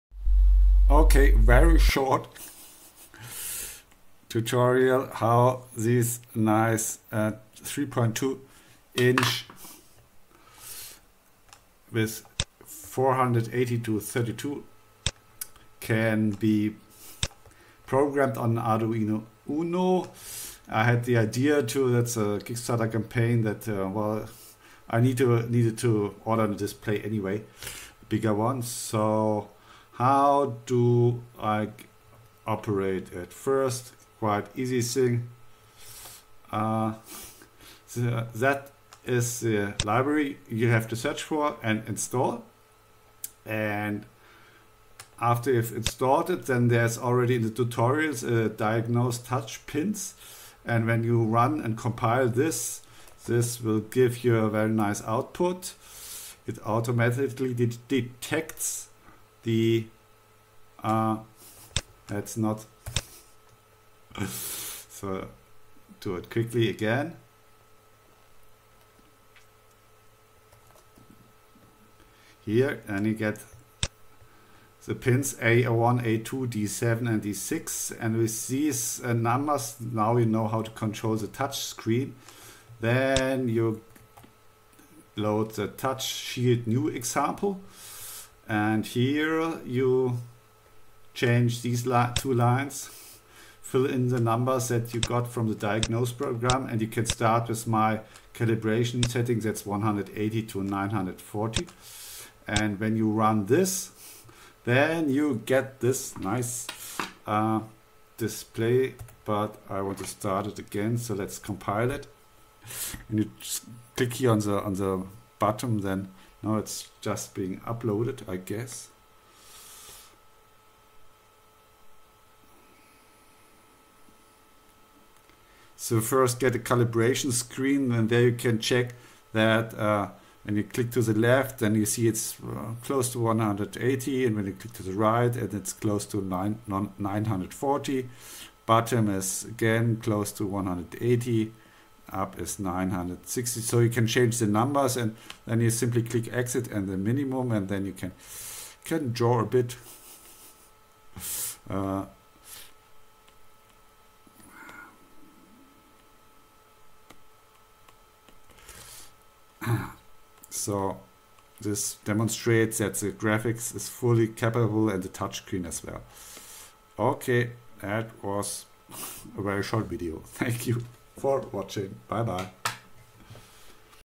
how to program cheap 480x320 3.2" Arduino Uno TFT-Touchscreen :-)
Took me a few hours to find the proper Arduino library.
But it is actually very very simple:

1. library-manager ... install "MCUFRIEND_kbv"

2. examples ... MCUFRIEND_kbv ... diagnose_touchpins
upload to Arduino Uno and take note of the serial output:

Diagnosing as:-
XM,XP: (A1, D7) = 32
YP,YM: (A2, D6) = 33

3. examples ... MCUFRIEND_kbv ... Touch_shield_new

change lines 12 and 13 to:

const int XP=7,XM=A1,YP=A2,YM=6; //320x480 ID=0x8357
const int TS_LEFT=180,TS_RT=940, TS_TOP=940,TS_BOT=180;

4. upload code and click left/right/top/bottom to confirm my 180/940 calibration.

5. click "exit" in the middle of the screen and have fun painting.

the 4 pints to read the resistive touchscreen are also used by the tft display:

A1 = LCD_WR
A2 = LCD_RS

(A0 = LCD_RD
A3 = LCD_CS
A4 = LCD_RST)

D6 = LCD_D6
D7 = LCD_D7

So if you want to support that display, these two pins must be able to read analog values:

A1 = LCD_WR
A2 = LCD_RS

The other two signal lines only need to read and write digital data:

D6 = LCD_D6
D7 = LCD_D7

graphics controller seems to be an ili9481